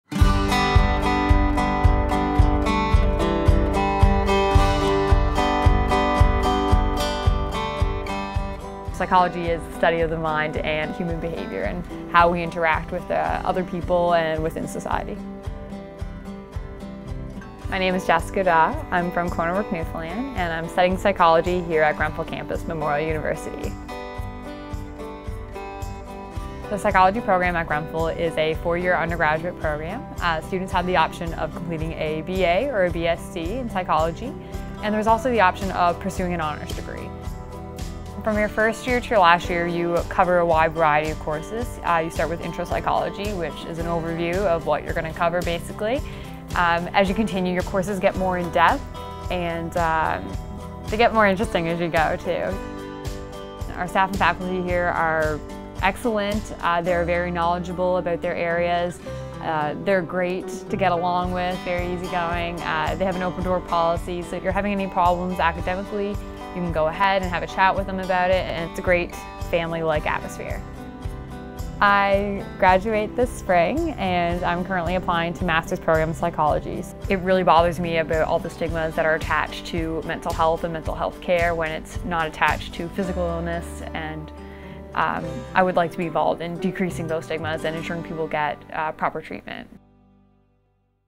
Grenfell Campus Bachelor of Arts or Science - Psychology
Psychology is the study of the mind and the study of human and animal behaviour. Are personality, abnormal behaviour and intelligence affected by genetics or the environment? Why do some people hate and others nurture? Can the unconscious mind influence behaviour?

Studying psychology at Grenfell Campus will give you skills and knowledge with lifelong relevance, particularly in the workplace: critical thinking, problem-solving and analytic abilities, effective writing and speaking skills. Hear what our student Jessica Dawe has to say.